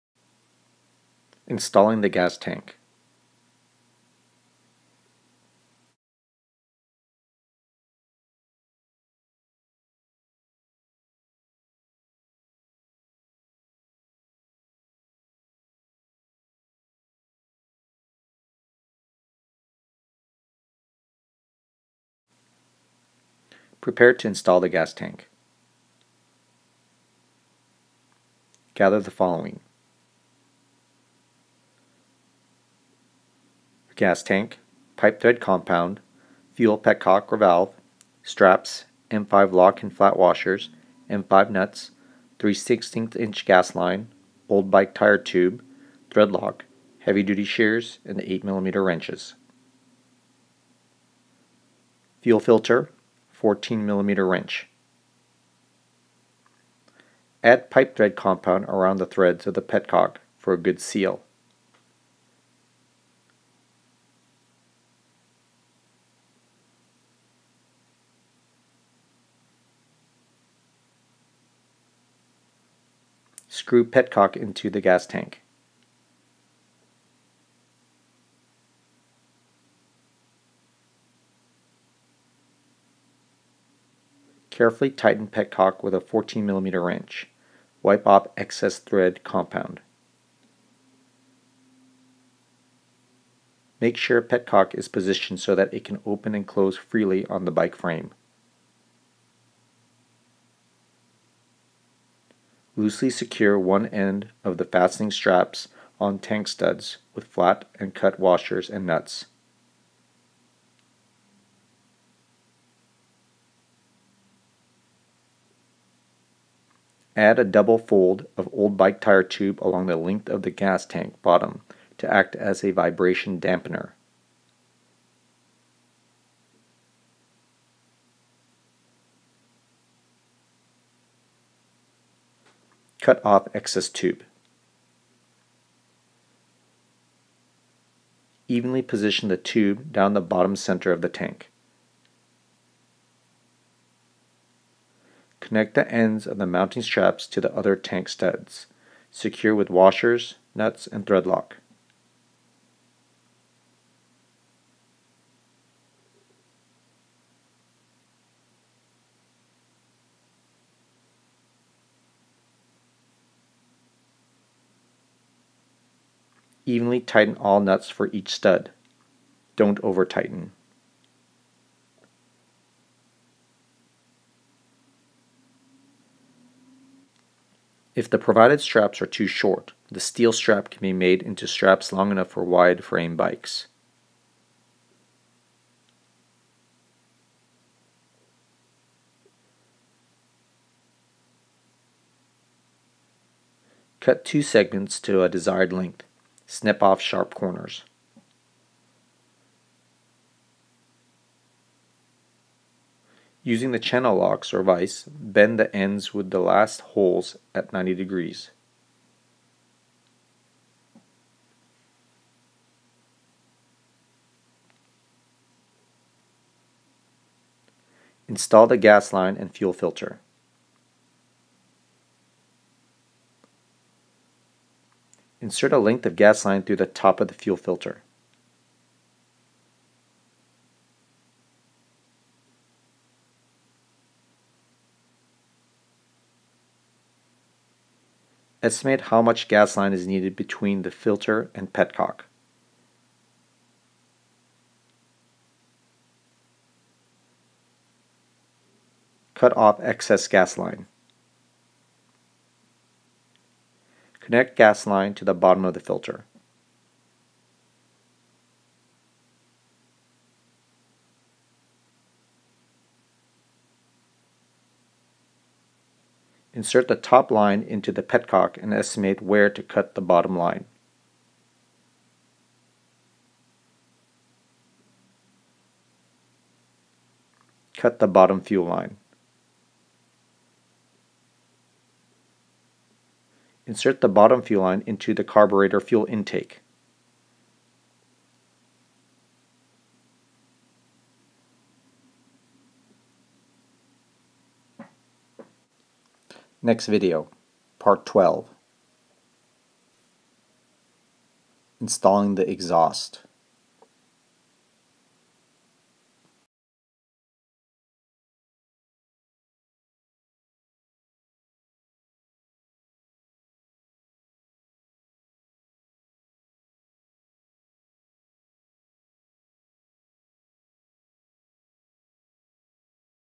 HOW TO MOTORIZE A BICYCLE-PART 11 (INSTALLING THE GAS TANK)
Part11
U-MOTO Motorized Bicycles presents a comprehensive video detailing the installation of a 2-stroke motor and its components on a cruiser bicycle.  HOW TO MOTORIZE A BICYCLE-PART  11 (INSTALLING THE GAS TANK)